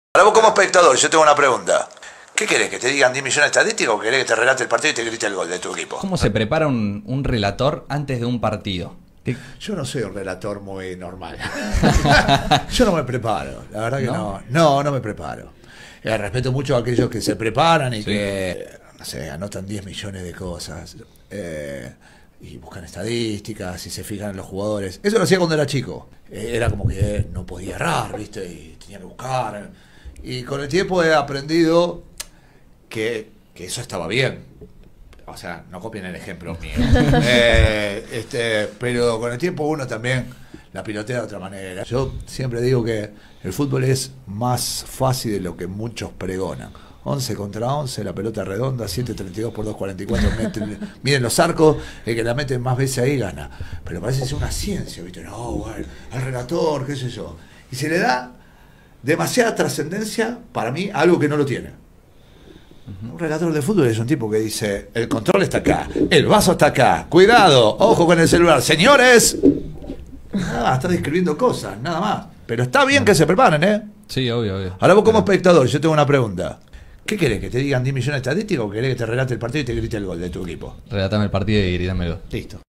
Estadísticas o grito de gol
Cuánto le importan las estadísticas en una transmisión deportiva a un espectador?